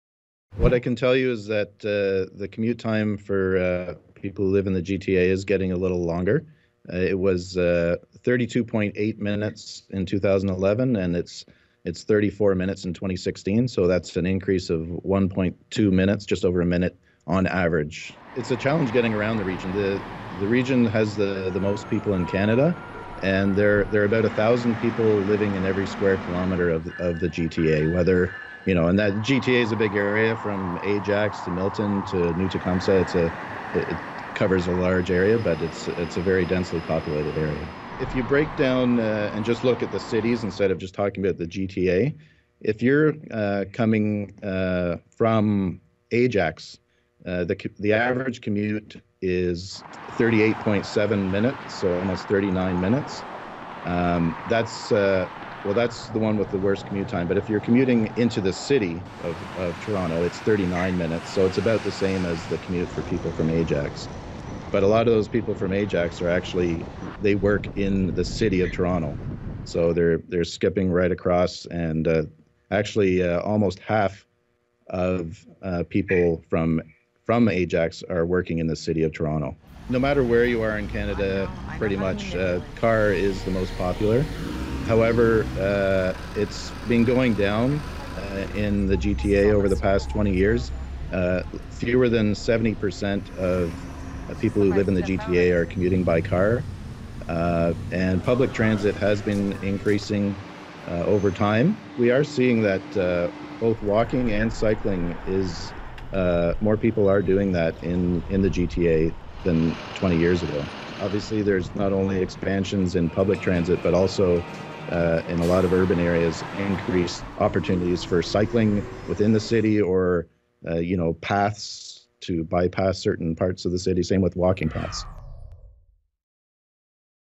GTA commute getting longer, sustainable transportation on the rise
Jason Gilmore with Statistics Canada breaks down what the latest census data says about commuting across the GTA.